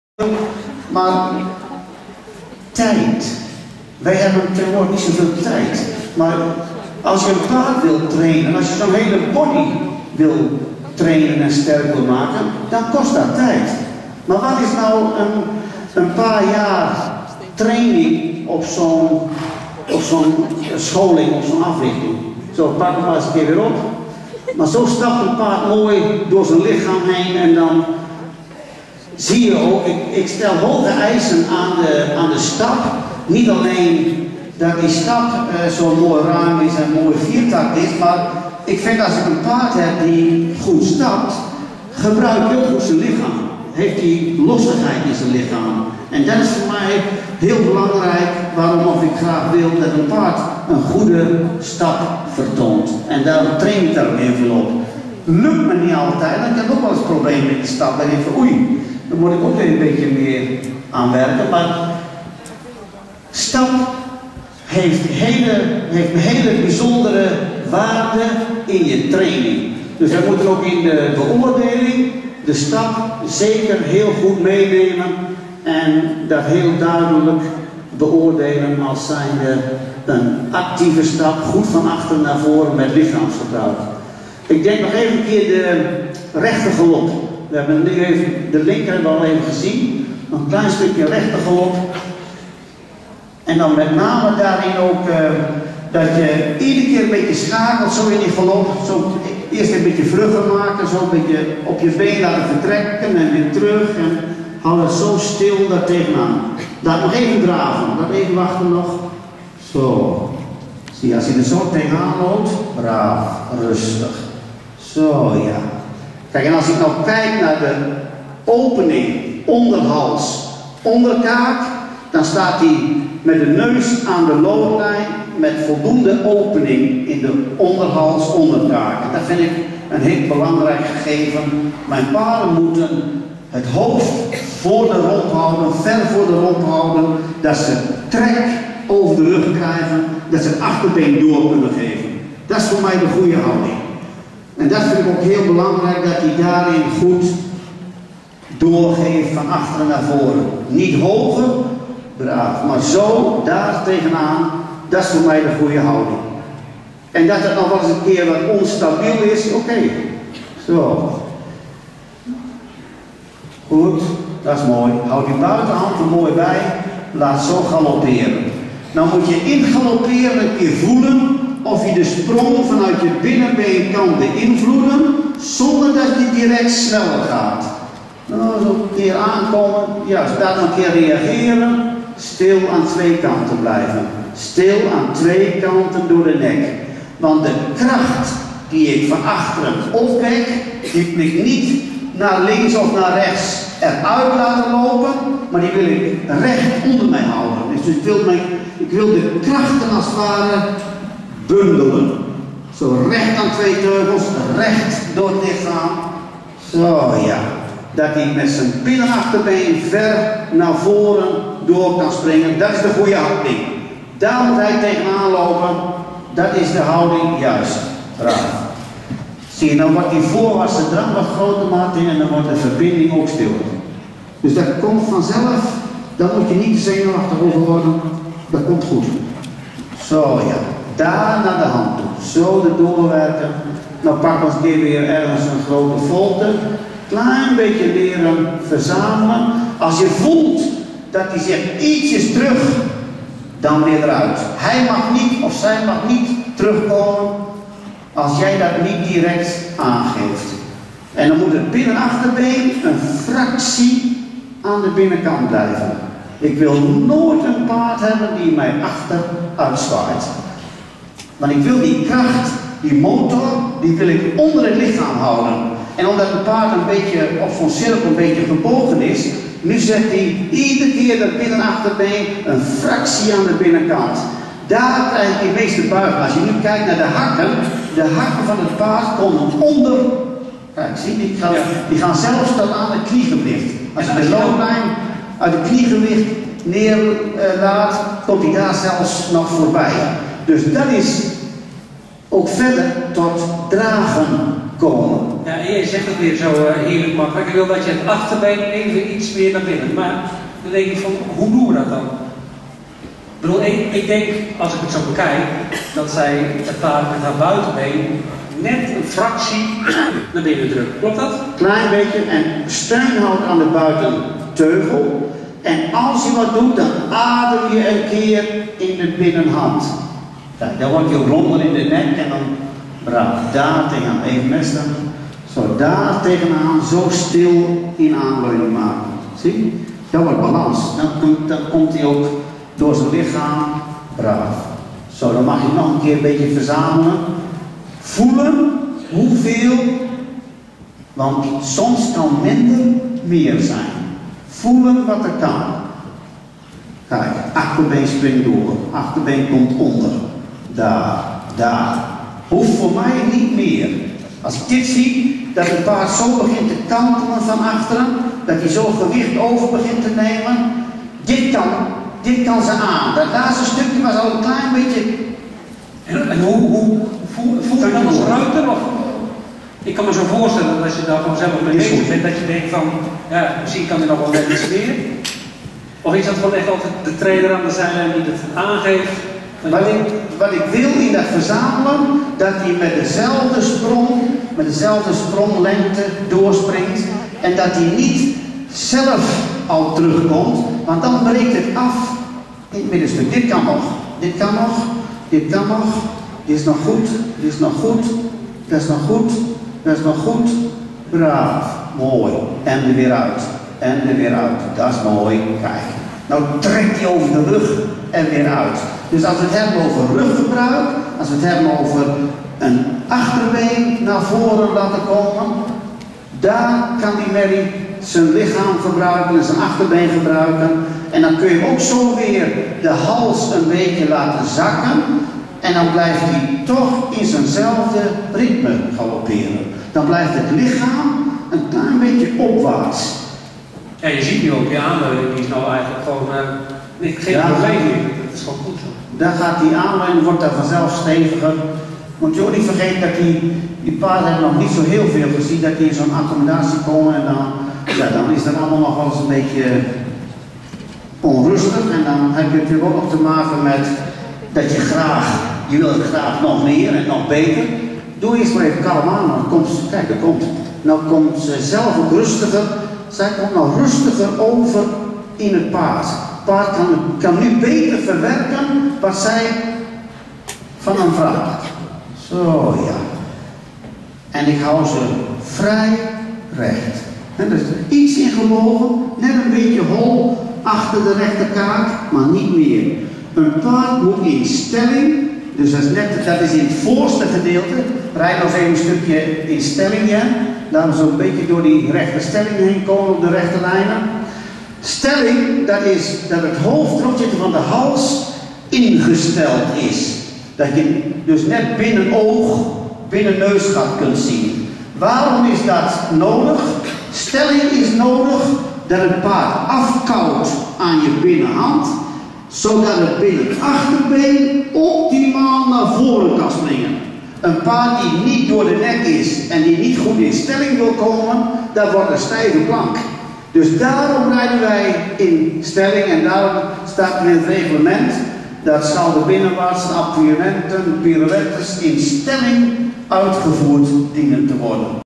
Amstelvreugd's Before Sunrise(Damon Hill)tijdens rondje instructie Ermelo deel II.
Clinic over het trainen van een dressuurpaard door Johan Hamminga en emmelie scholtens tijdens Rondje Instructie te Ermelo.

Gereden door :Jennifer Sekreve